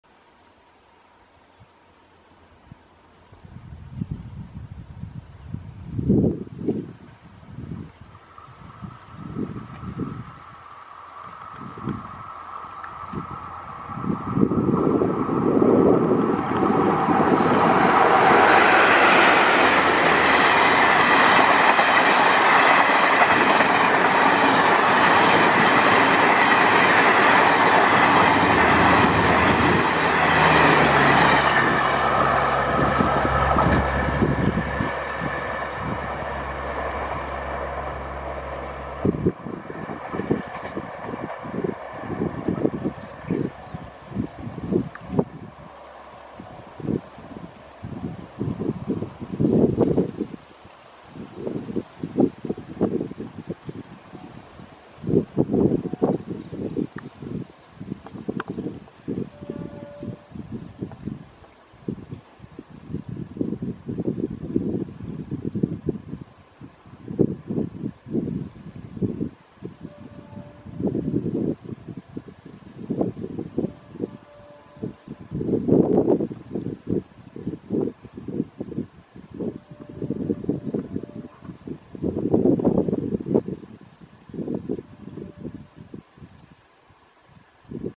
Fastest train between Allahabad & Mumbai with OFFLINK HWH WAP4 | ICF Duronto Express
Bi-weekly 12294 Allahabad-LTT 'Duronto Express' appears at Thansit behind an unusual link #22364 HWH WAP4.

The regular link for this train is ET WAP4. This Duronto uses ICF's ex-Rajdhani rake now owned by C.R.